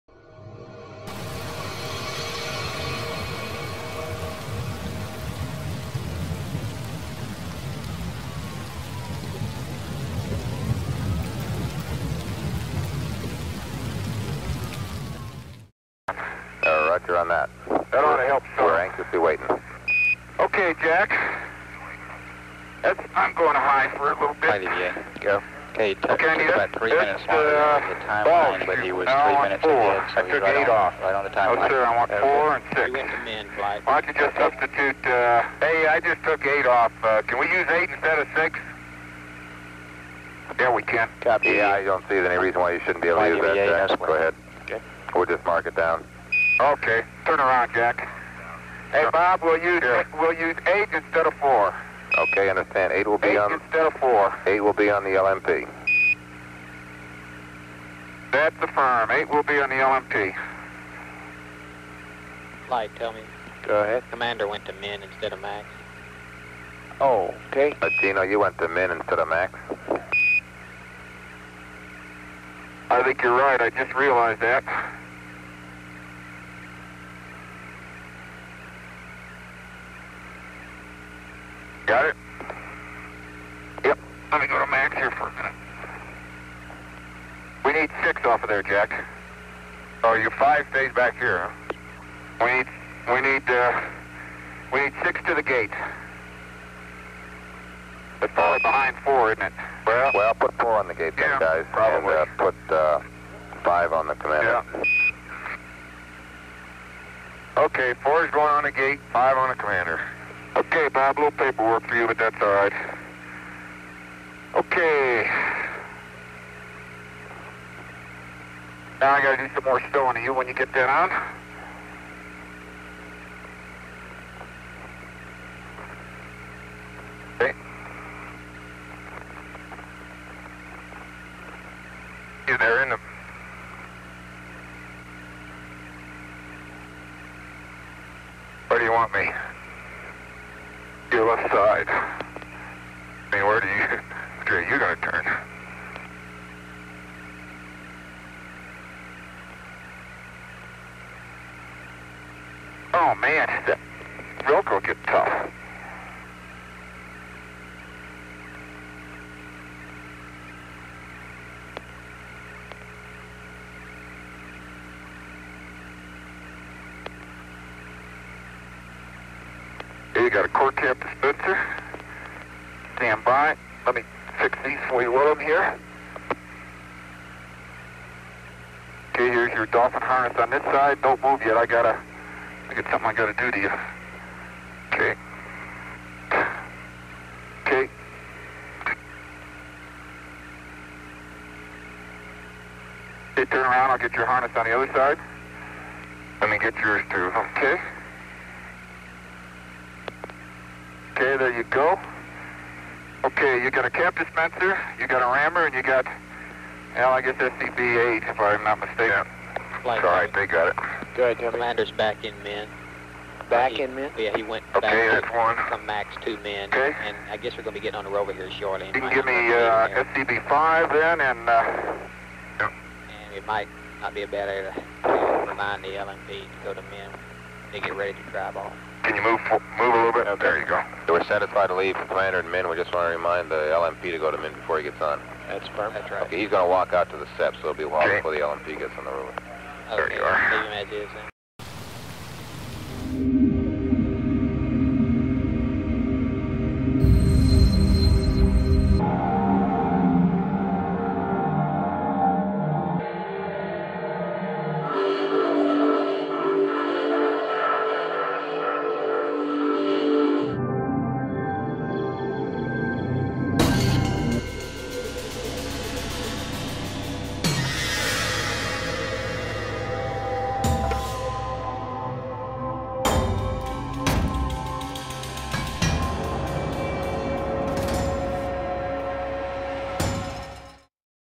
Moon rocks & soil Apollo 17 - a17v.1411419 - 069
The First Half of the Traverse to Geology Station 2. This Apollo 17 video contains at least three scenes in which the side of the mountain behind the astronauts is partially visible. There may be some problem with the video technology used, but it would be very strange to create a similar defective image in these three different situations.

These videos have a great value being
original and unedited.

Please note the rocks and the other objects

Observe how the surface is easily messed up
once stepped on and the interesting objects
thus being revealed.

Please, observe the many objects which look more
like silhouettes of houses or buildings.